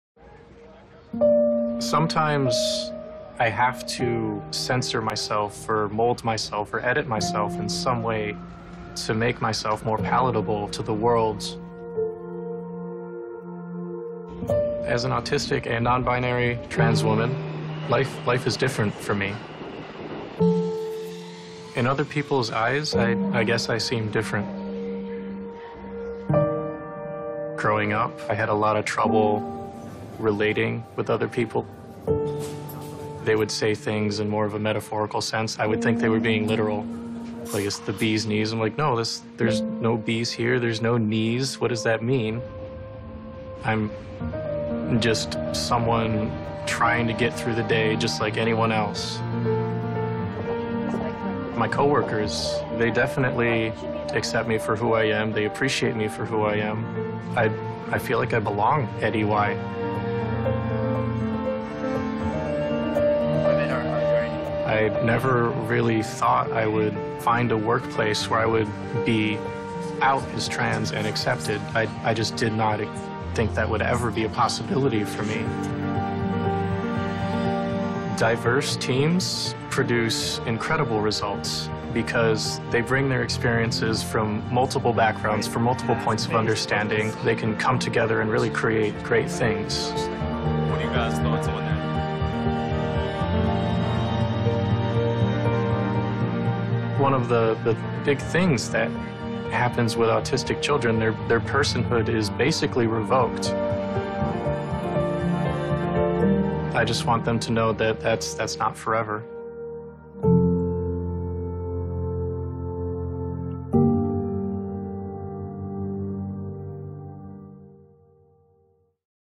Meet Kyle.
What happens when great minds don't think alike? At EY, we believe that diverse teams generate the best results. Watch Kyle's story.